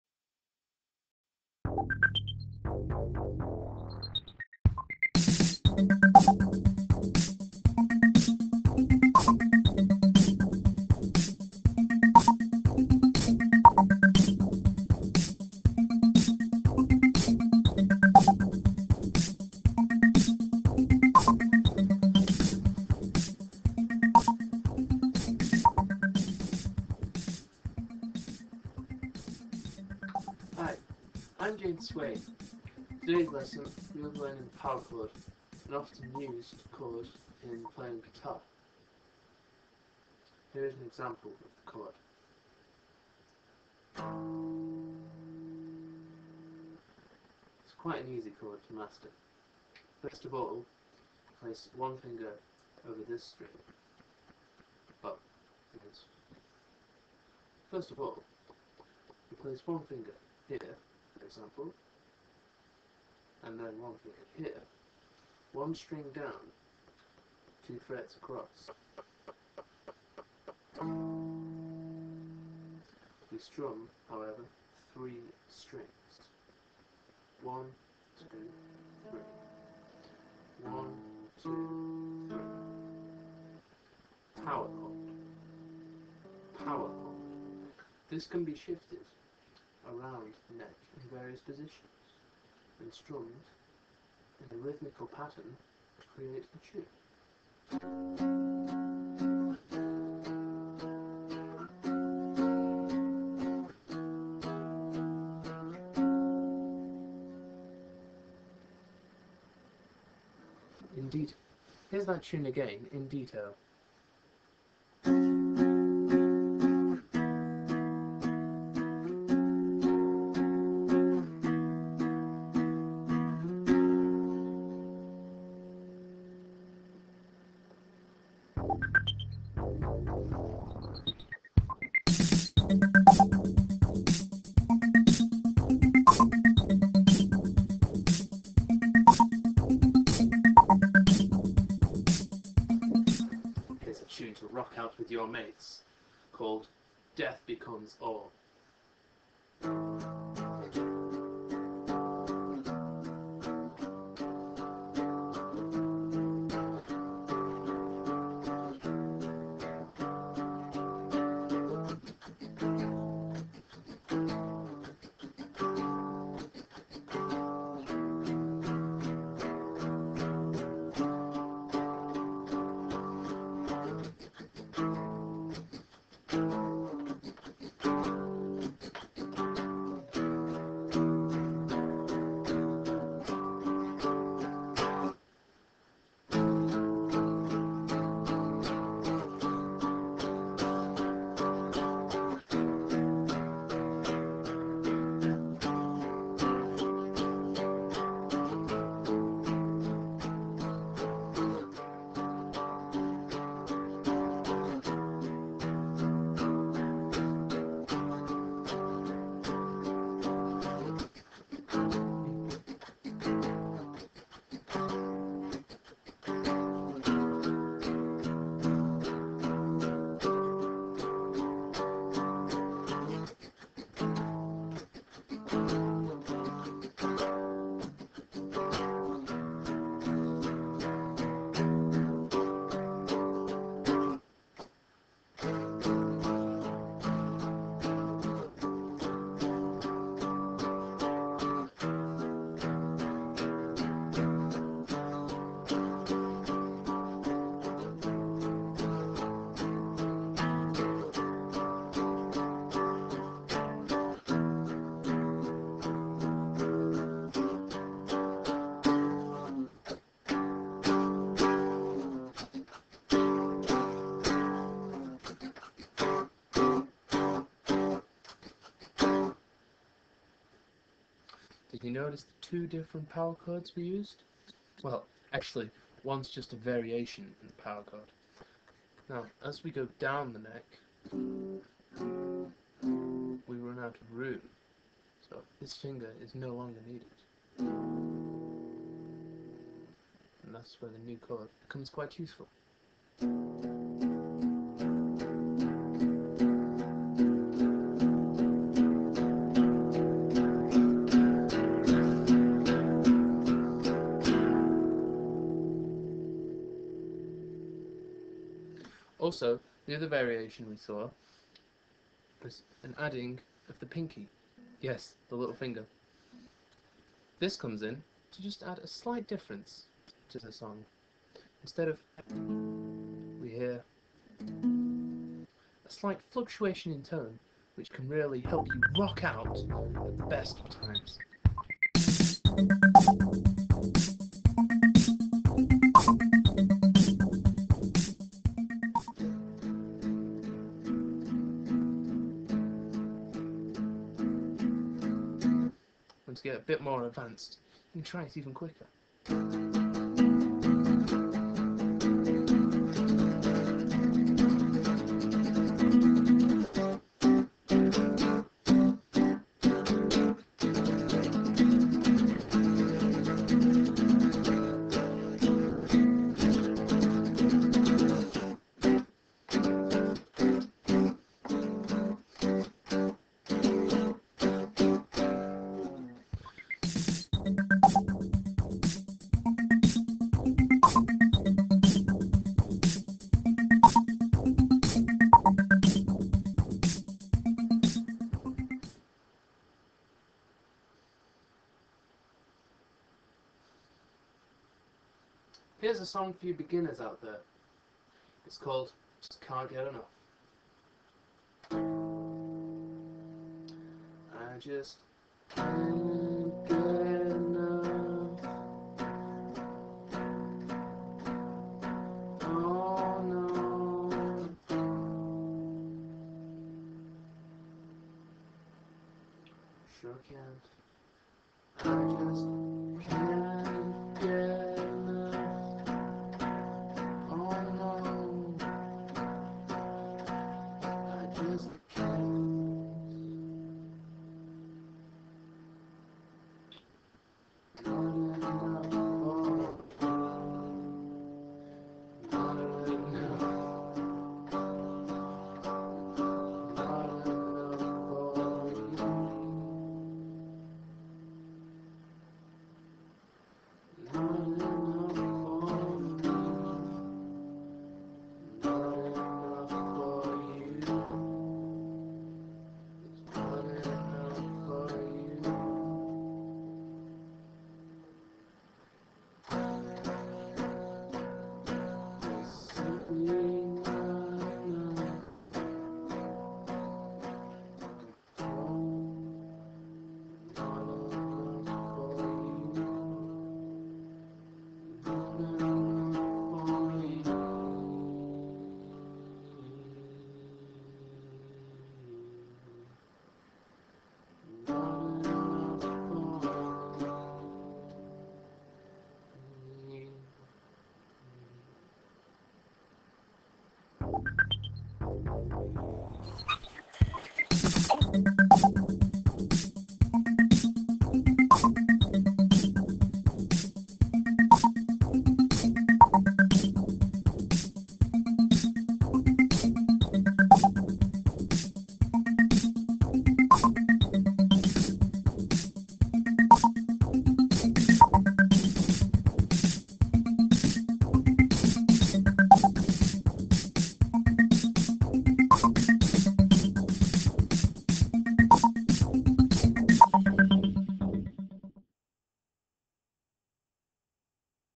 Guitar Techniques
A guitar tuition video I made as a skit of a really bad retro tuition video I bought from a charity shop.